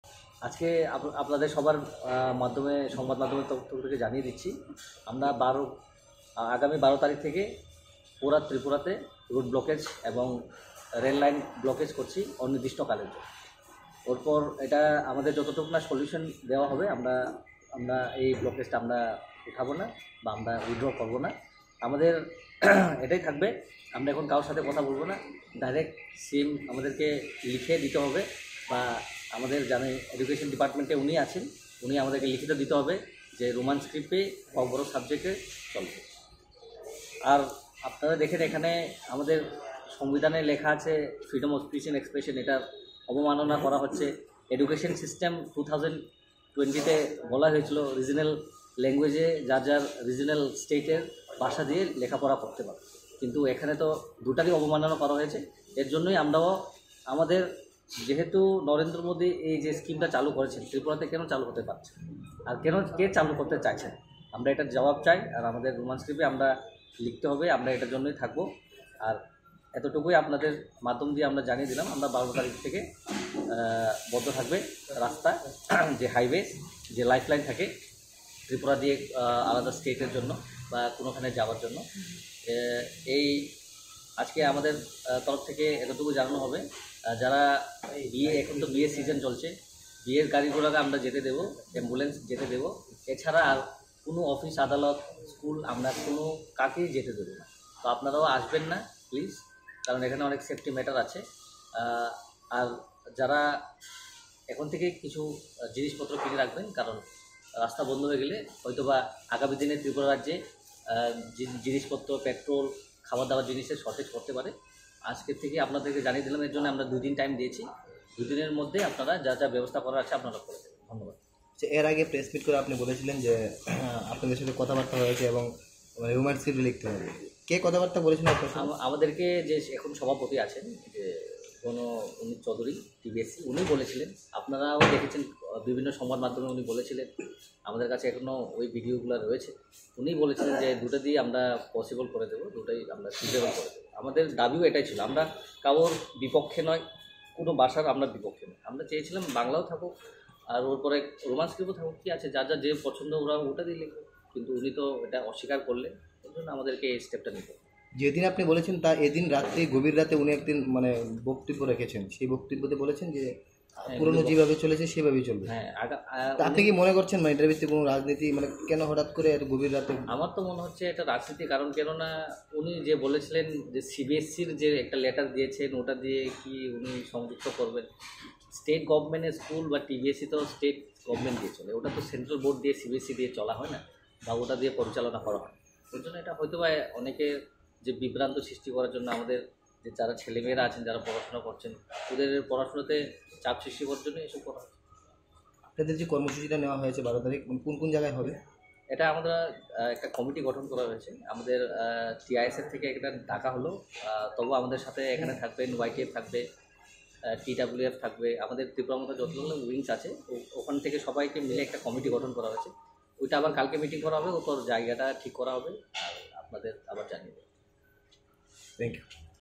রোমান স্ক্রিপ্ট এর দাবিতে ত্রিপুরা বনধের ডাক ১২ ই ফেব্রুয়ারি | Planet Tripura News
Tripura bandh called on February 12 to demand Roman script
Audio Credits:-
Planet Tripura News